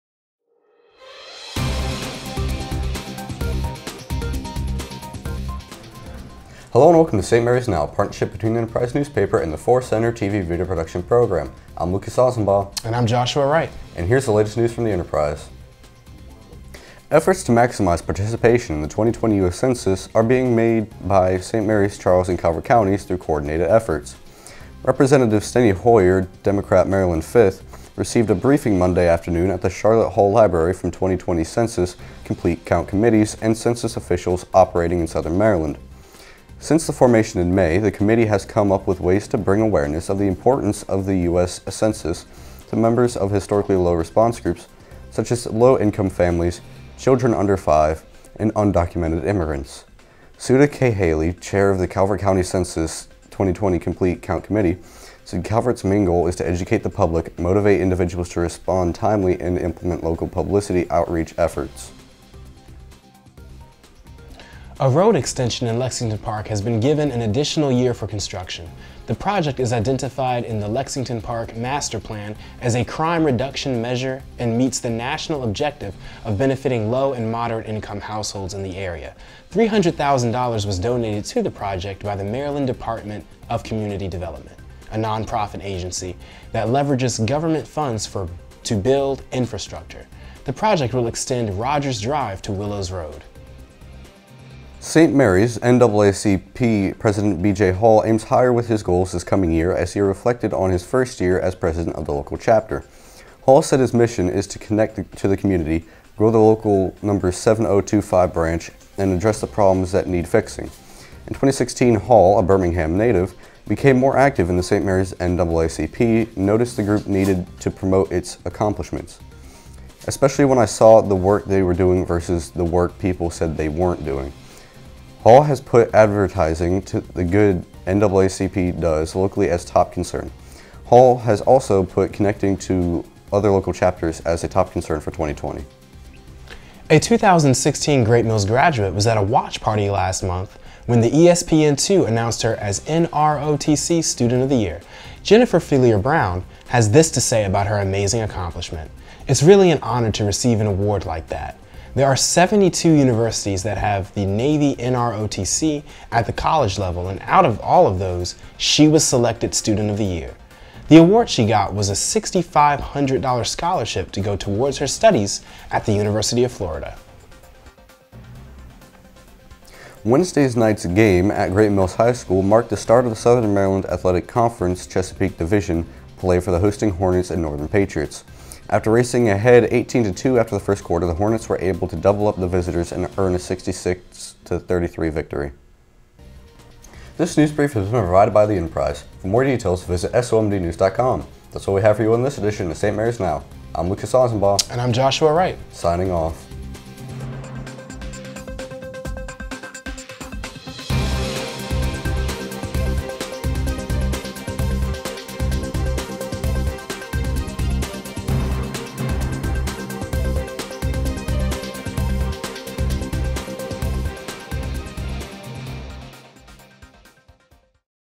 St. Mary's Now (from 1-10-2020)
St. Mary's Now is a partnership between The Enterprise newspaper and the Dr. James A. Forrest Career and Technology Center's TV/Video Production program. This newsbrief is produced by Gold Team and covers stories from The Enterprise's Friday, January 10, 2020 edition. Stories include: Already-busy NAACP president aims higher; Extension to road gets extra funding; Briefing on census participation given; Great Mills grad named NROTC student of the year; and a look at local sports. Luke Ausenbaugh and Josh Wright are your anchors. Emily Bennett is your director. Sarah Baldacchino is your editor.